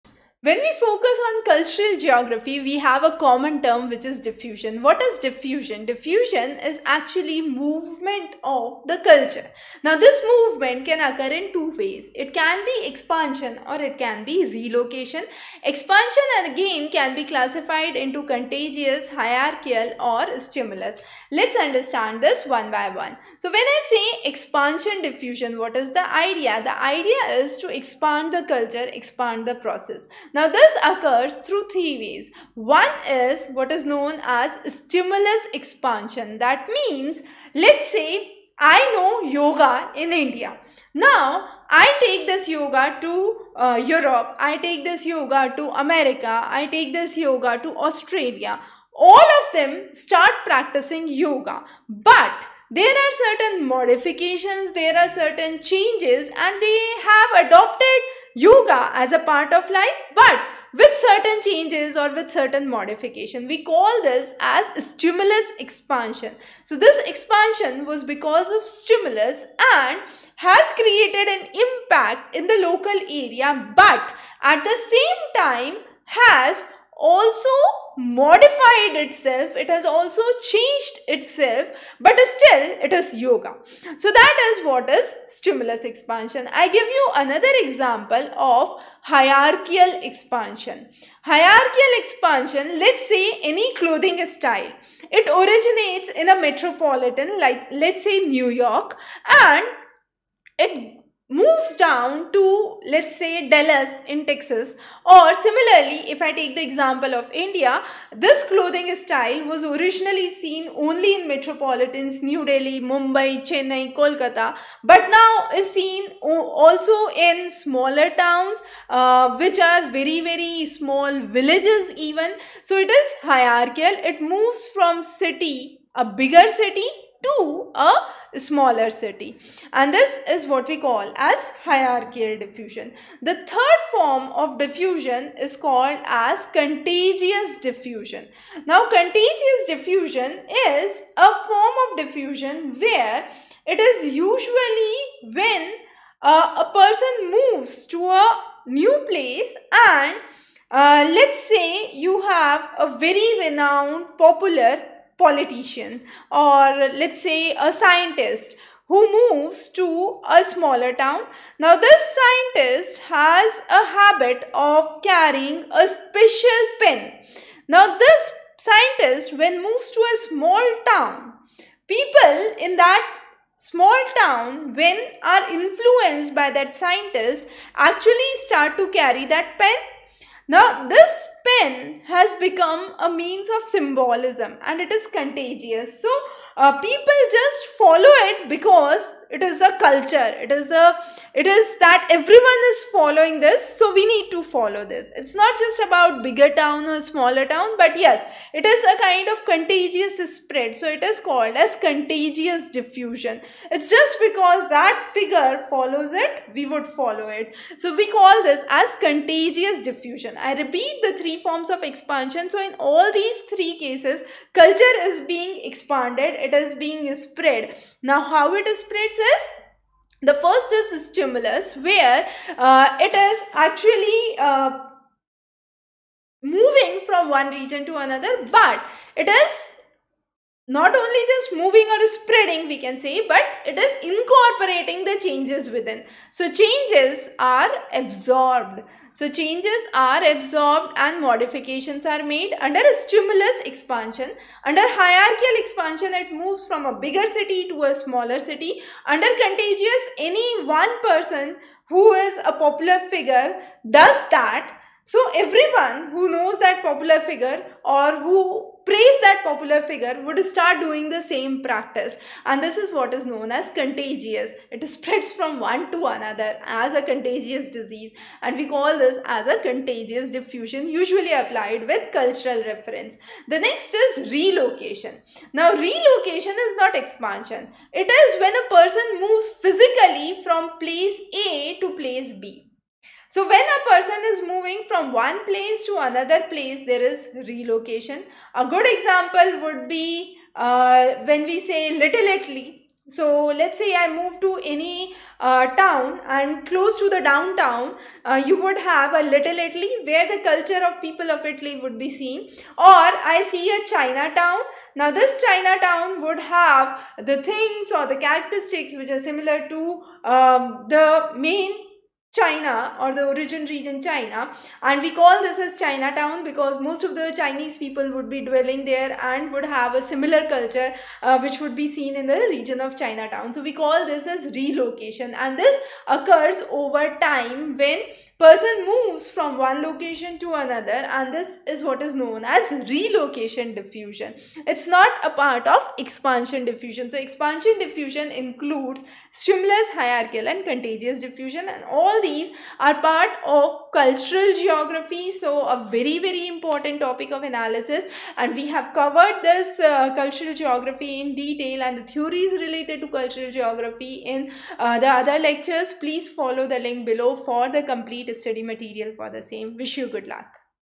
Diffusion: Expansion-Contagious, Stimulus, Hierarchical; Relocation|Cultural Geography|AP Geography
Chapters:
0:00 Diffusion
0:30 Expansion Diffusion
1:06 Stimulus Expansion
1:30 Hierarchical Expansion
2:22 Contagious Diffusion
4:35 Relocation